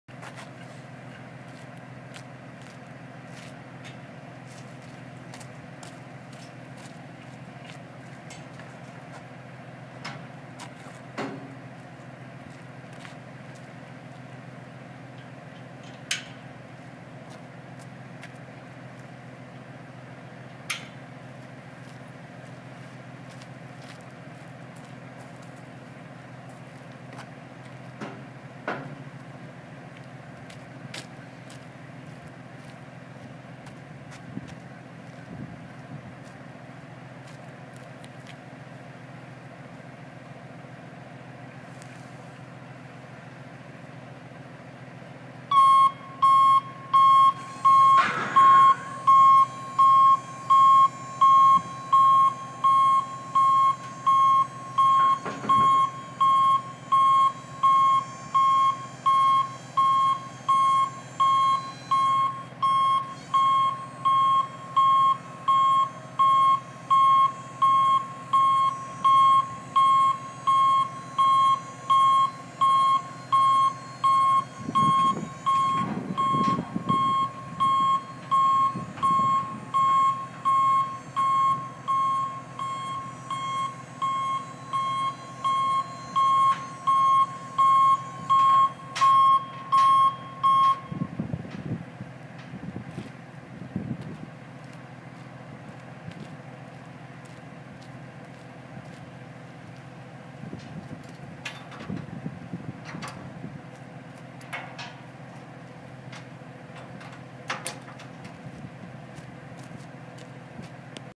Neville Built combine trailer fold up
Fully hydraulic combine trailer folding up video.